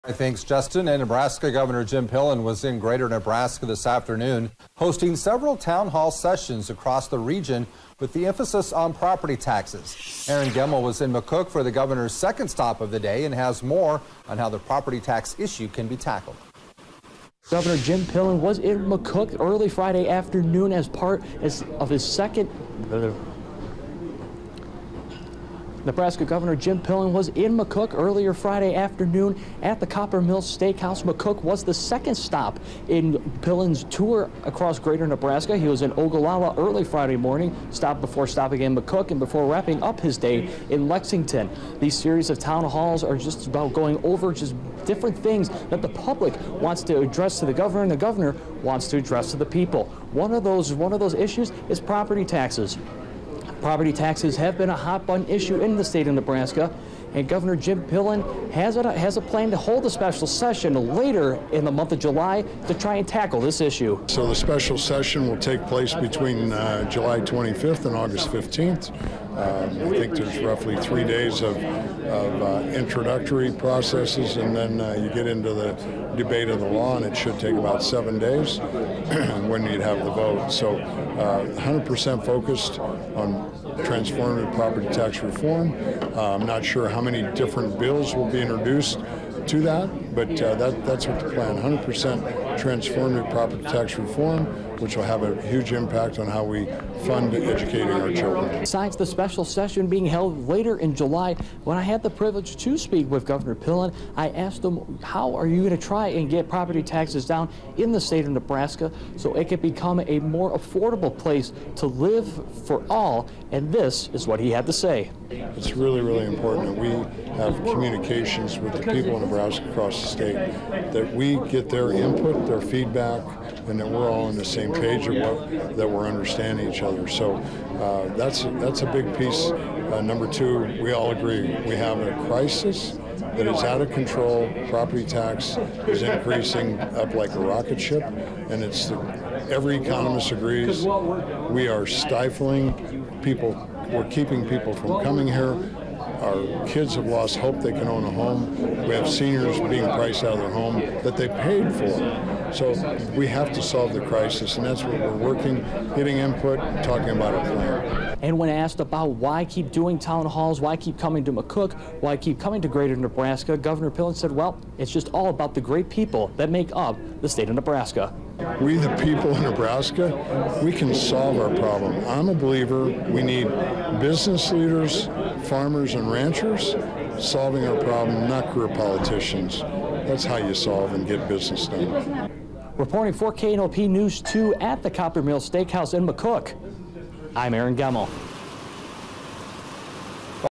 Governor Jim Pillen visits greater Nebraska to discuss property tax crisis
Governor Jim Pillen was in McCook early Friday afternoon at Coppermill Steakhouse to discuss property taxes along with other state-wide issues.